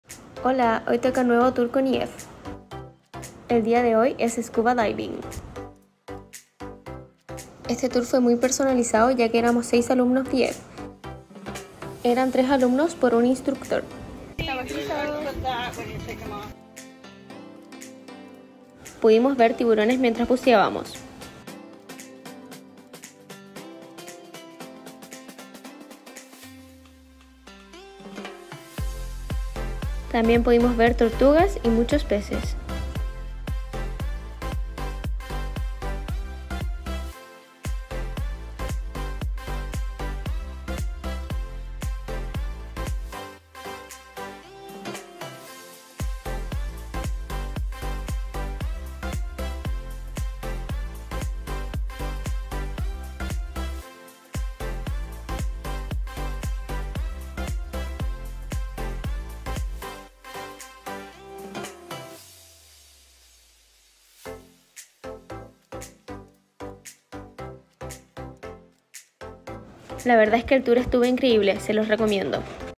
Scuba Diving | Honolulu, Hawaii🌺
Hola! Soy Fernanda Sagás. Actualmente me encuentro de intercambio con EF en Honululu, Hawaii.

Si quieres tener más información sobre mi experiencia acá te dejo mis redes

EF Ambasador Chile, Hasta 200 usd de descuento si viajas de mi parte!

Scuba Diving lo reserve a través de las actividades que realiza EF en este destino, lo recomiendo porque van más alumnos de EF y hace que puedas conocer aún más personas, adicionalmente para mi era más seguro que todos fueran alumnos de EF porque me daba más confianza y también tenía la seguridad en caso de cancelación del tour no me iban a estafar ya que la reservación era a través de EF.

Si vienen a Honolulu, Hawaii con EF tienen que reservar esta actividad!